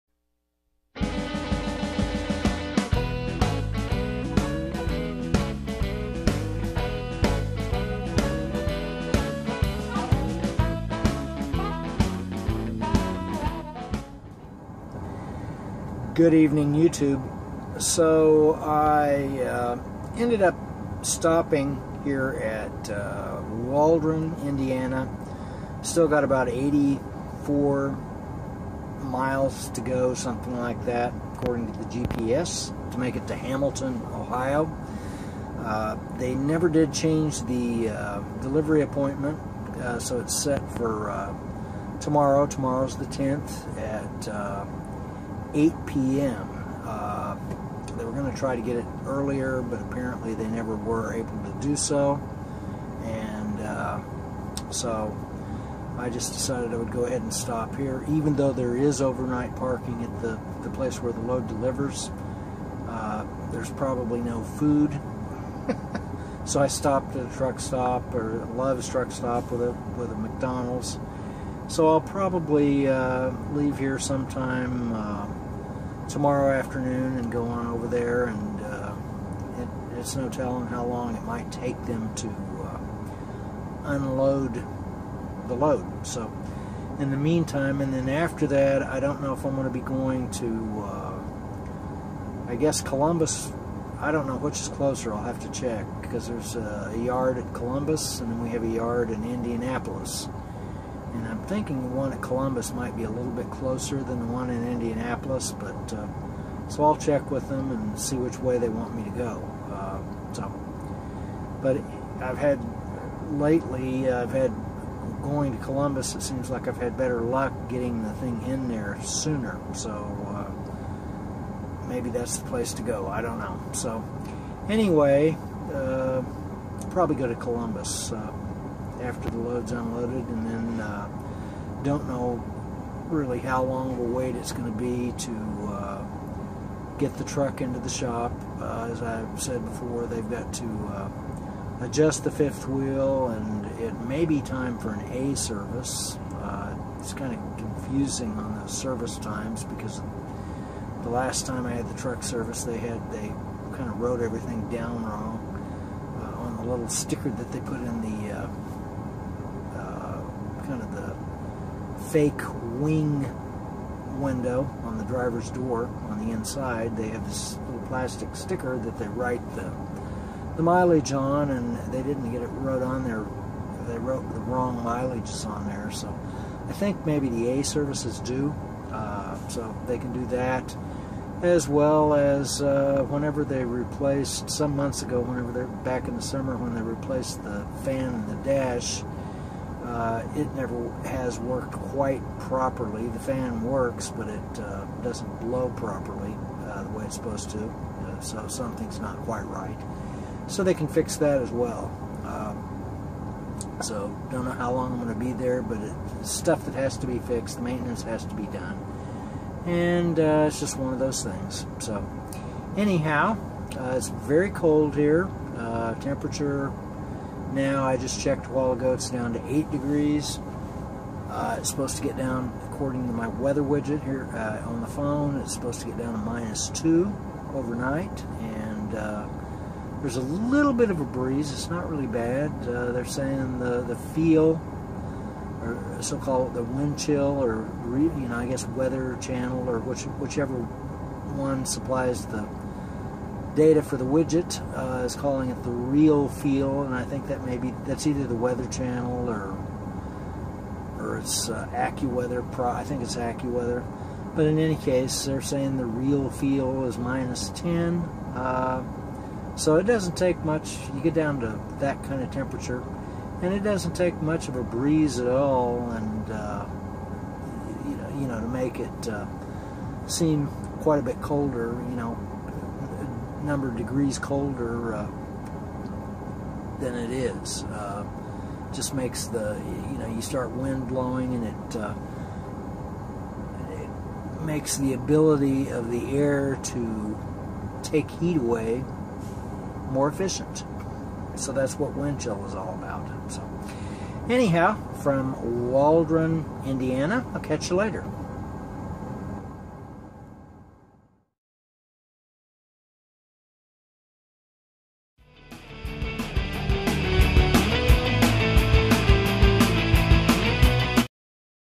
TRUCKING WITH TOM: EPISODE 241: Waldron, Indiana
Im waiting to deliver Saturday at 8:00PM in Hamilton, Ohio.